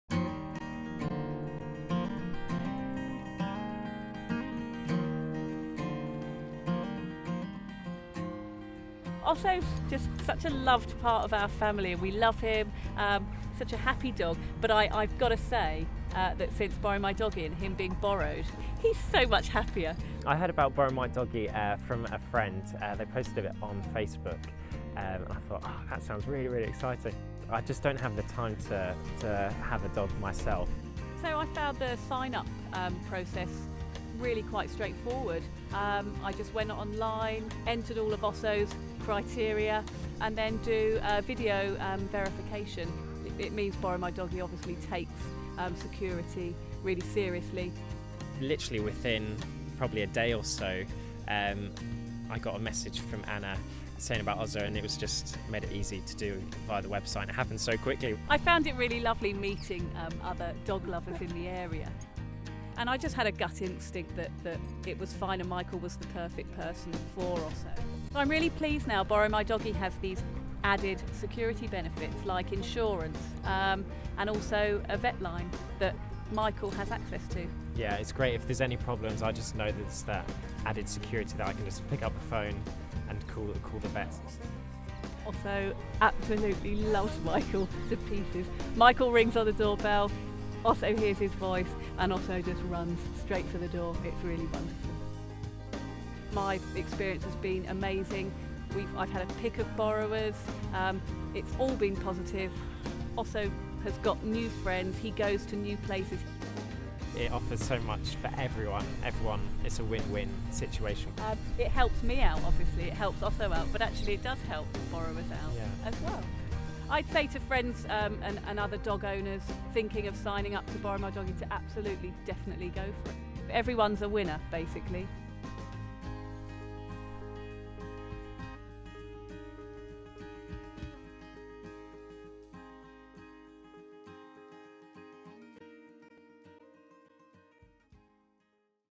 BorrowMyDoggy Waggy Tales: Oso, Anna and Michael
▶︎ Introducing BorrowMyDoggy's first ever Waggy Tales video - high paws! Our members share the loveliest stories, so what better way to bring them to life than with a heartwarming video.

Put your paws up and get ready to smile as owner Anna and borrower Michael talk about their pawfect match with Labrador Oso. As they say: "Our experience has been amazing, nothing but positive."

BorrowMyDoggy matches dog owners with local borrowers for walks, sitting and holiday care.

Thanks to Oso, Anna and Michael.